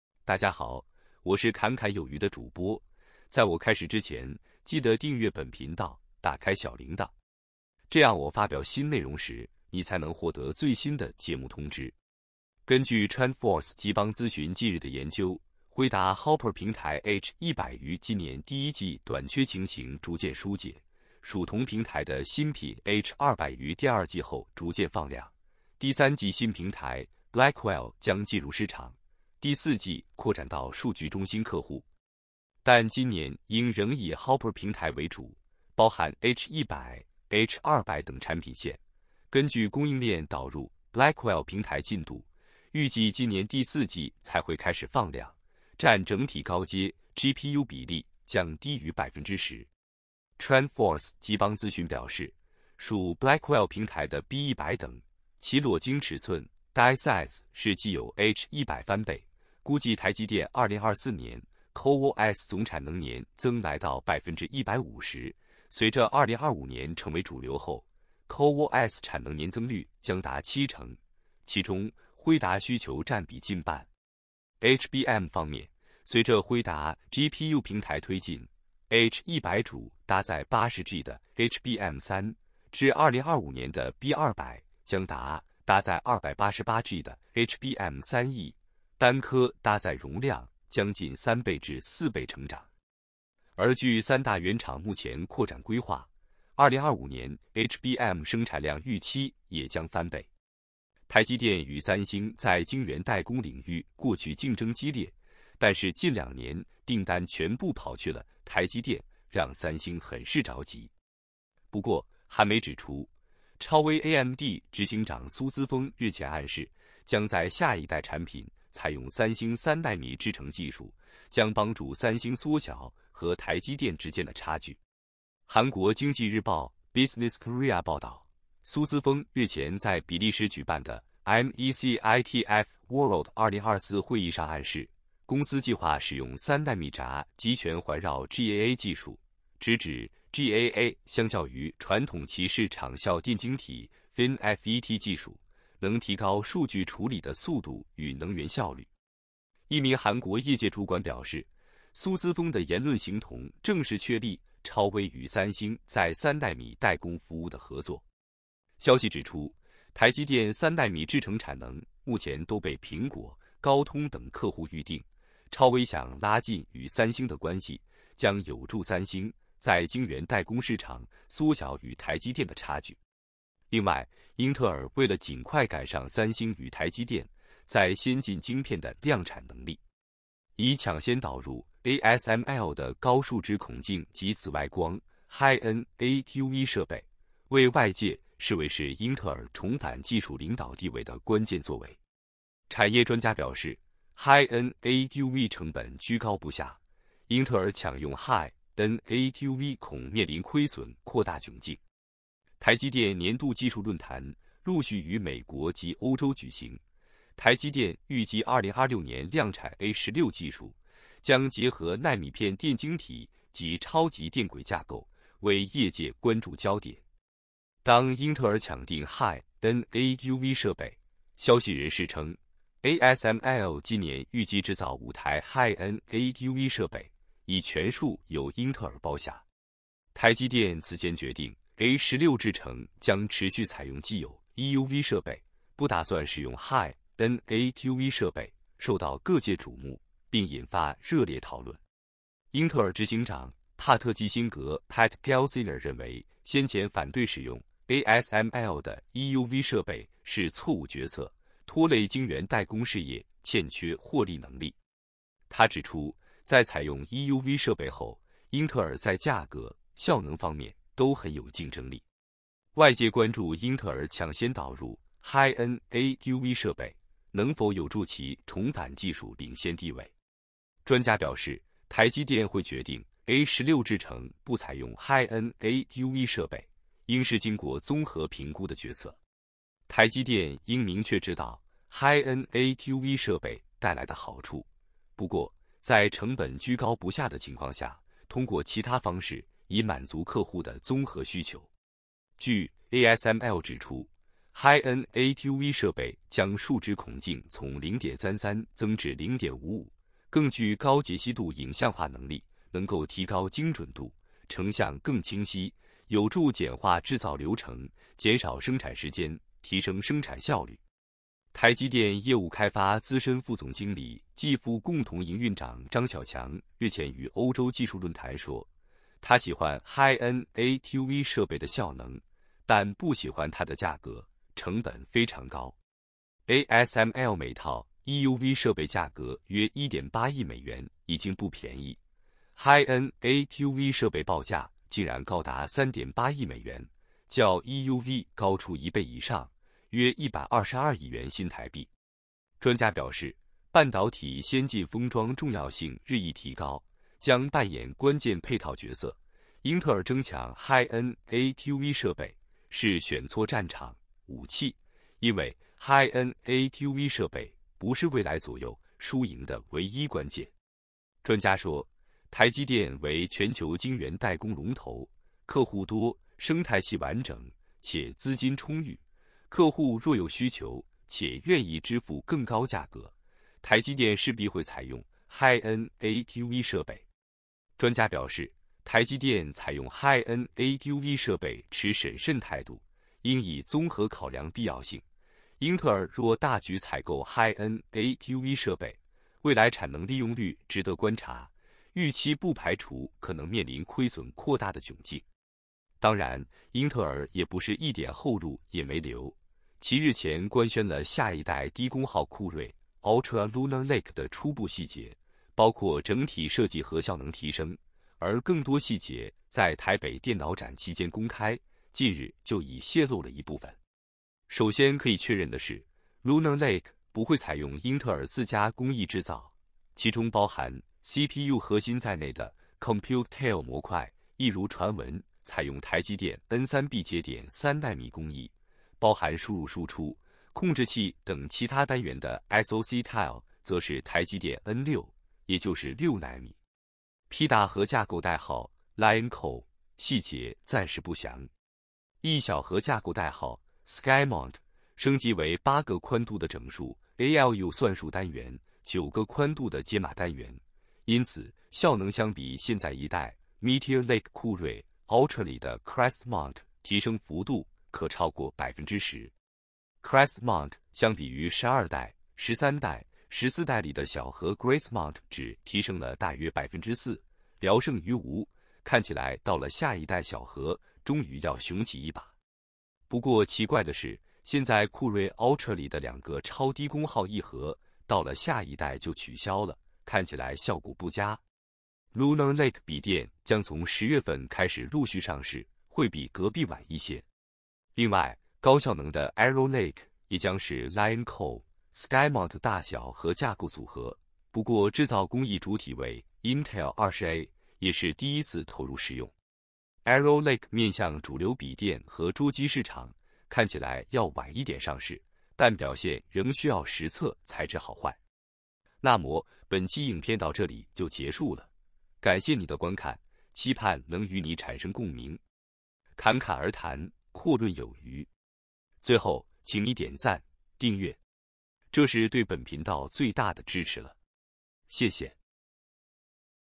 反向買瘋！英特爾全新CPU竟慘棄自家20A工藝，台積電N3B能效實在太炸裂了。破規矩Lunar Lake用上兩種製程，第2代酷睿Ultra規格實錘。韓媒竊喜大客戶AMD，變心要轉向三星3奈米。
TrendForce集邦咨詢表示，屬Blackwell平台的B100等，其裸晶尺寸die size是既有H100翻倍，估計台積電2024年CoWoS總產能年增來到150%，隨著2025年成為主流後，CoWoS產能年增率將達7成，其中輝達需求佔比近半。另有消息指出，台積電3nm制程產能目前都被蘋果、高通等客戶預訂，超威想拉近與三星的關係，將有助三星在晶圓代工市場縮小與台積電的差距。台積電採用High-NA EUV設備持審慎態度，應已綜合考量必要性，英特爾若大舉採購High-NA EUV設備，未來產能利用率值得觀察，預期不排除可能面臨虧損擴大的窘境。英特爾Lunar Lake不會採用英特爾自家工藝製造，其中包含CPU核心在內的Compute Tile模塊一如傳聞採用台積電N3B節點3nm工藝，包含輸入輸出、控制器等其他單元的SoC Tile則是台積電N6，也就是6nm。